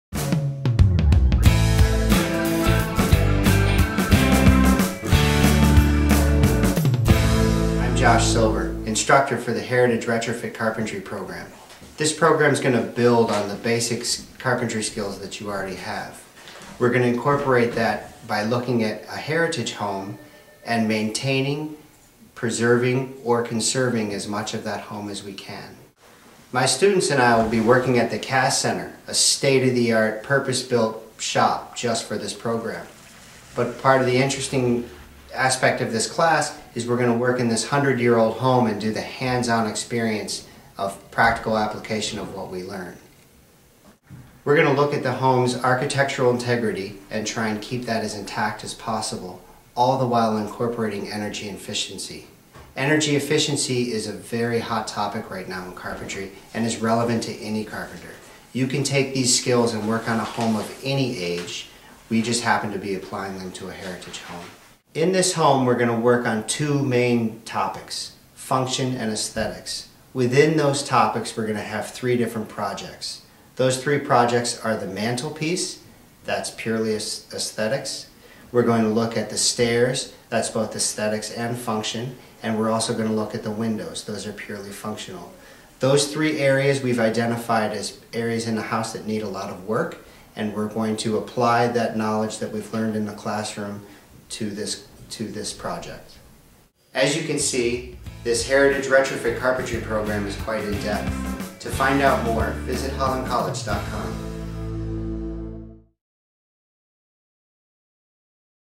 Heritage Retrofit Carpentry program at Holland College
Josh Silver, instructor for the Heritage Retrofit Carpentry program at Holland College talks about the projects that he and his student will undertake.